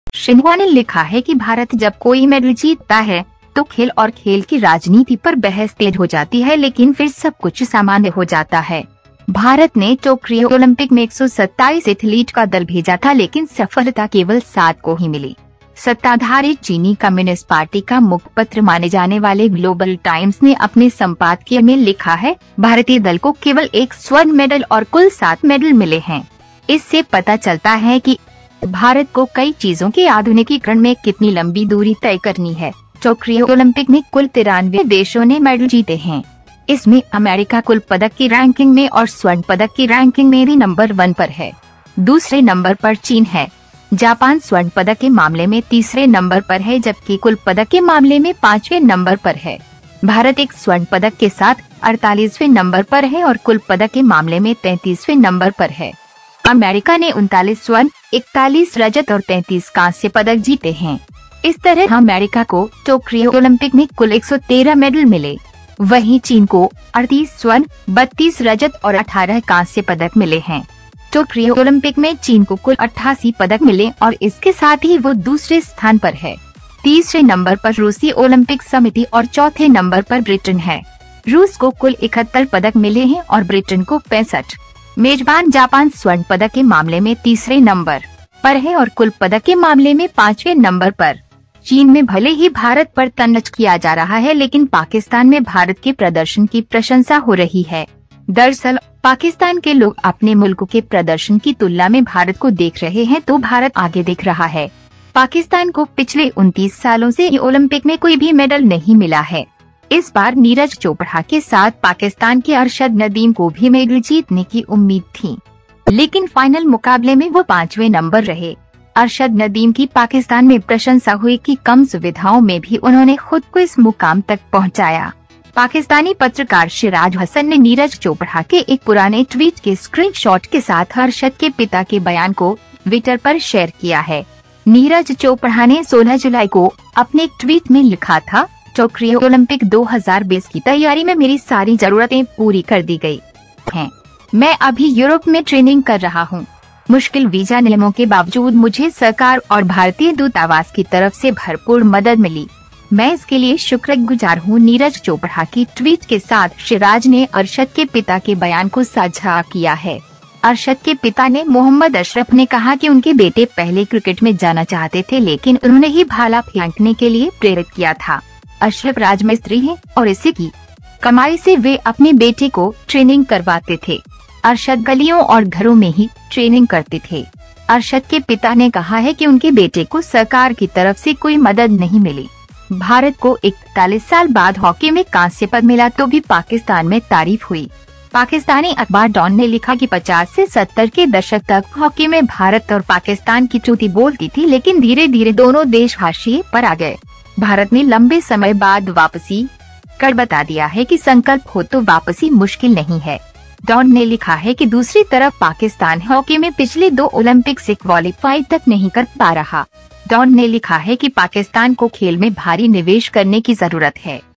ओलंपिक में भारत के प्रदर्शन पर चीन का तंज तो पाकिस्तान में हो रही तारीफ....
टोक्यो ओलंपिक में भारत के प्रदर्शन को लेकर चीनी मीडिया में भी काफी चर्चा है. चीन की सरकारी समाचार एजेंसी शिन्हुआ ने एक लेख छापा है, जिसमें तंज किया गय चीन और पाकिस्तान ऐसे दोस्त हैं, जिनकी नीति भारत के खिलाफ एक जैसी मानी जाती है. भले भारत और पाकिस्तान में व्यापक सांस्कृतिक समानता है लेकिन चीन पाकिस्त...